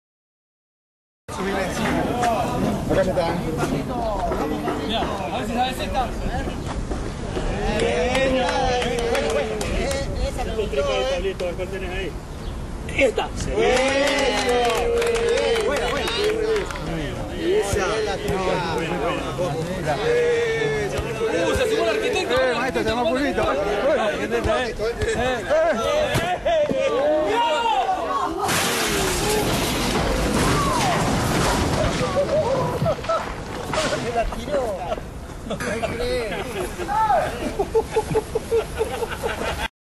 Engineer vs mason Jefa playing soccer
best engineer in the world playing soccer with a mason